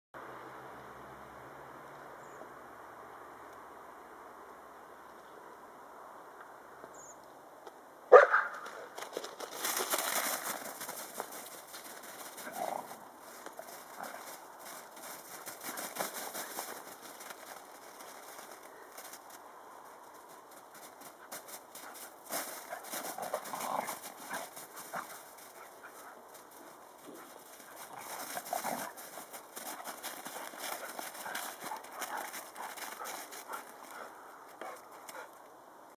Lekker uitdagen....
Sneeuw in de tuin. Fenne is haar moeder Sanne weer eens lekker aan het uitdagen. Eind december 2014